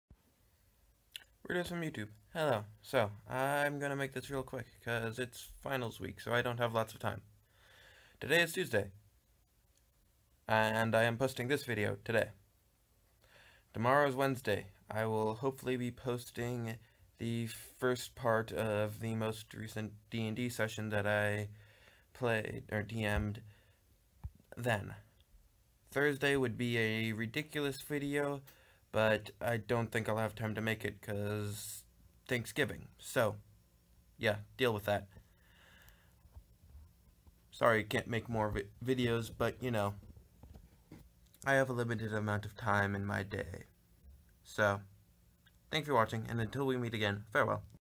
S1E12 thAnksgiving week video schedule
today's tuesday video merely says when more videos will be up sorry to be boring, but at least I'm sideways. yes it's technically Wednesday now, but the bus had terrible wifi so I was unable to do my uploading then
Also have any ideas for NPC names, leave your suggestions in the comments section